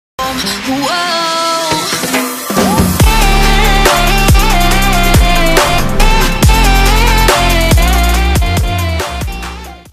Intro per l'utente Pro Scout - Animazione fatta con Blender
Pro Scout

L'utente in questione chiese al sottoscritto di fare una intro carina per il suo canale.
Comprendendo il suo stile e vedendo i suoi game play che ha caricato, ho subito pensato a una intro simile a questa.